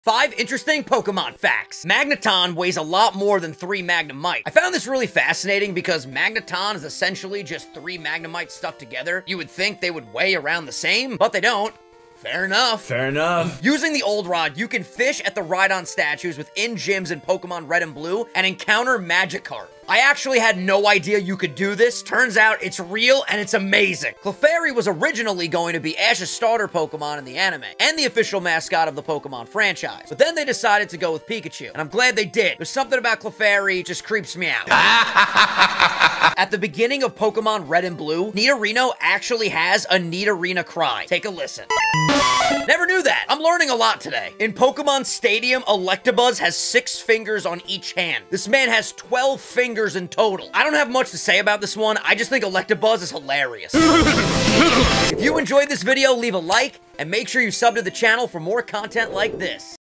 5 Interesting Pokémon Facts!
In this video I list 5 very interesting facts about Pokemon!